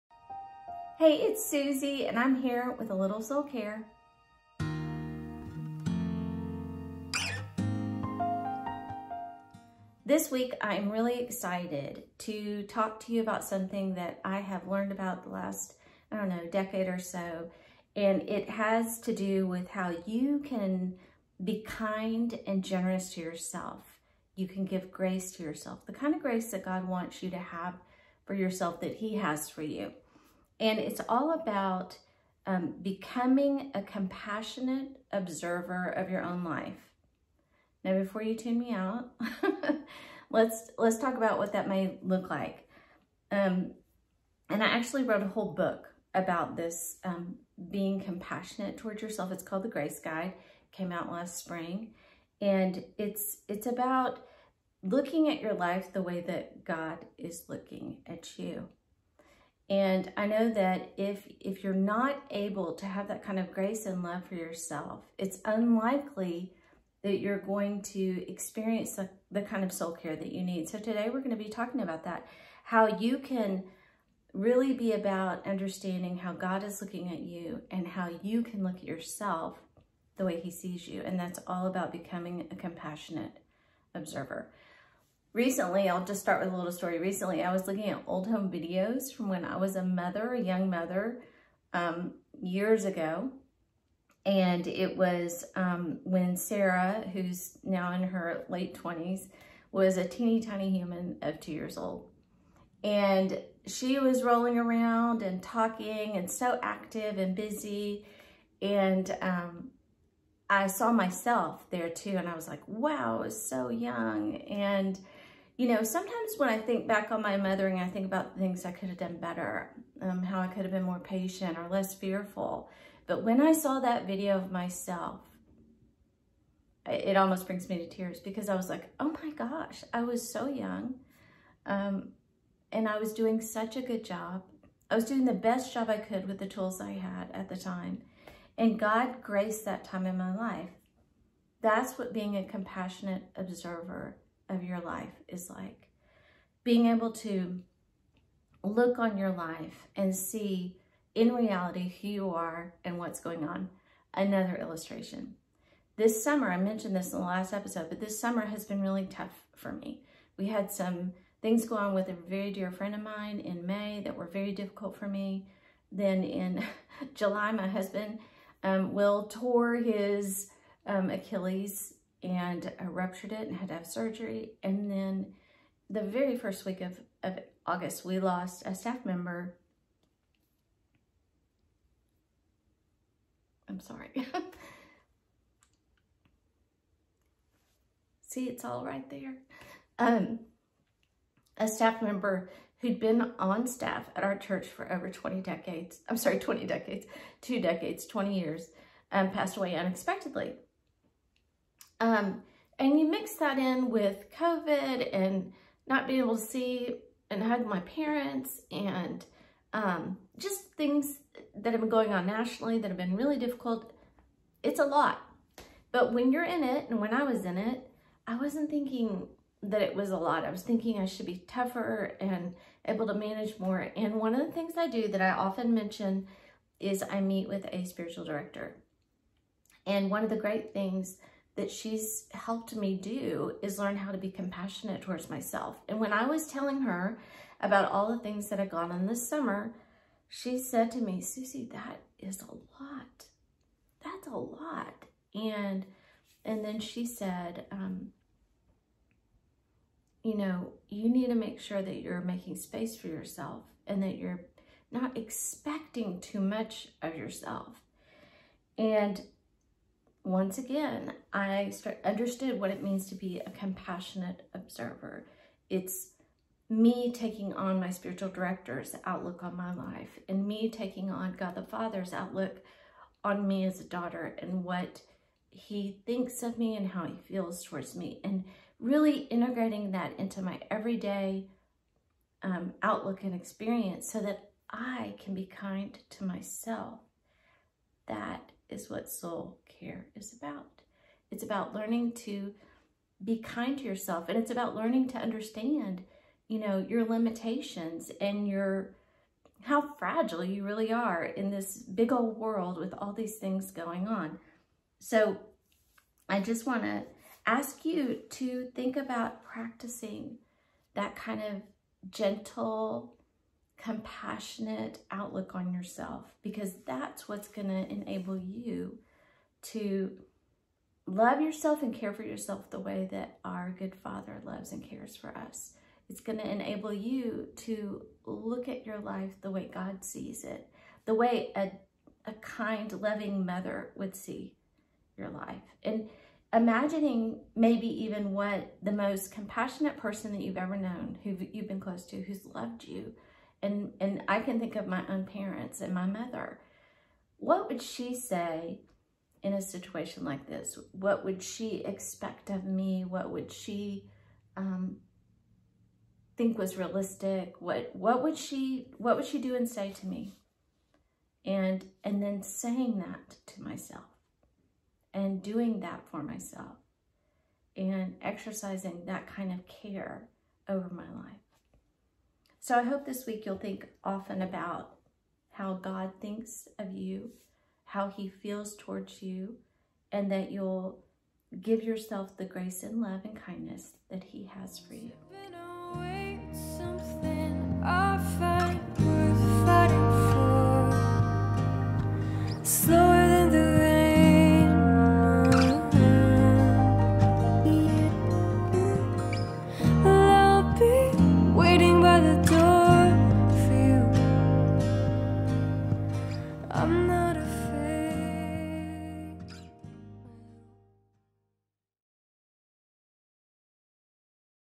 Soul Care Saturday | September 19
What does it mean to be a compassionate observer of your own life? In today's Soul Care with Susie Davis, we'll be encouraged to love and care for ourselves as God cares for us.